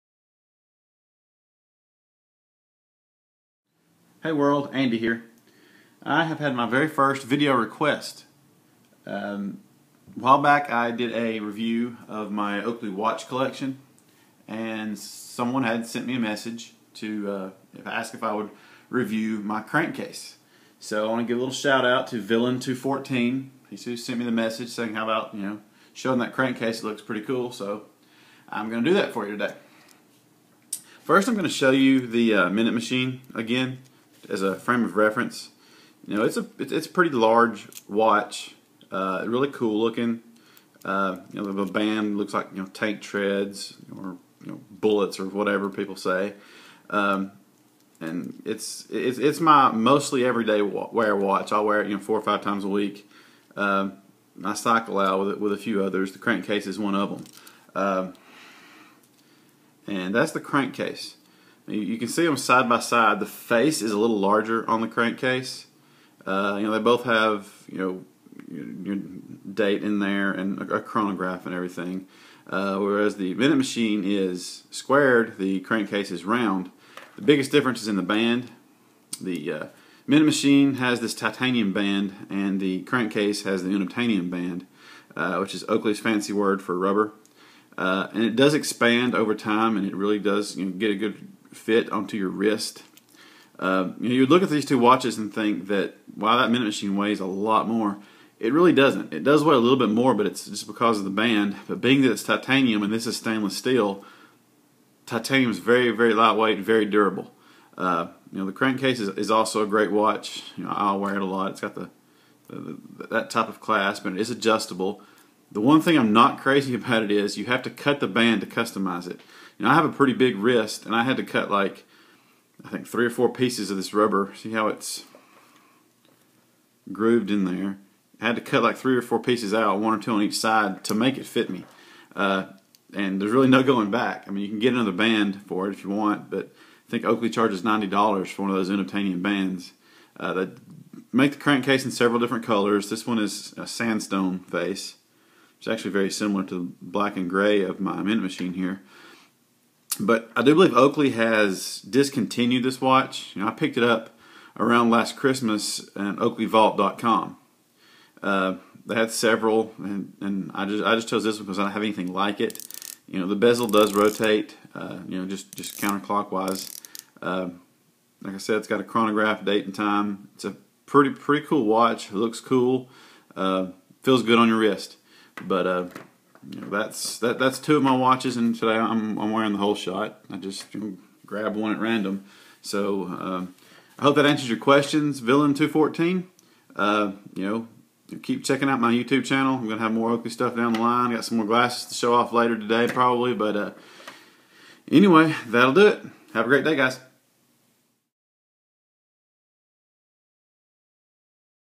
Oakley Crankcase Review
This is my review of the Oakley Crankcase watch. It is in reply to a request from villan214. Enjoy!

I apologize for the grainy quality.